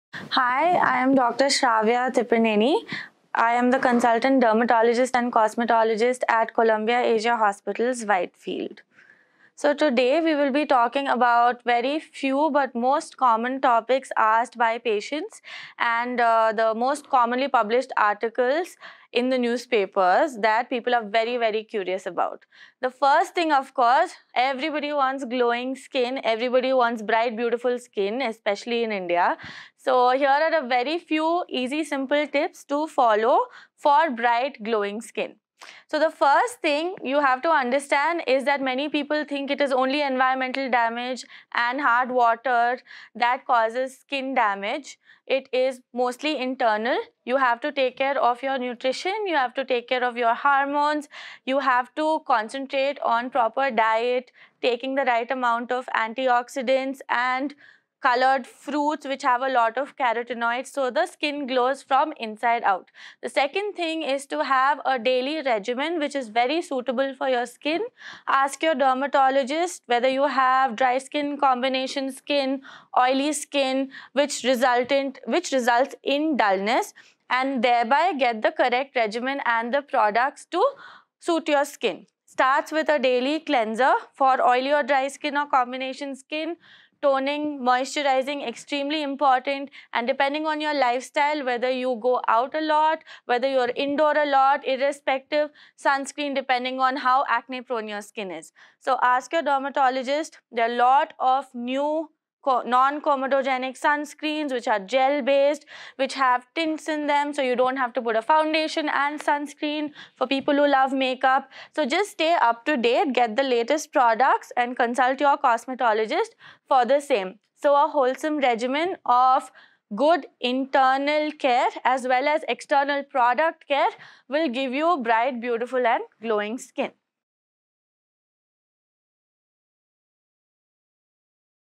Tips for bright, glowing skin | Dr. Sravya Chowdary Tipirneni, Columbia Asia Hospitals, Whitefield
To achieve bright, glowing skin, one must take care of their nutrition and hormones as part of a daily regimen. Consulting a dermatologist would help guide you to the correct products to apply on your skin.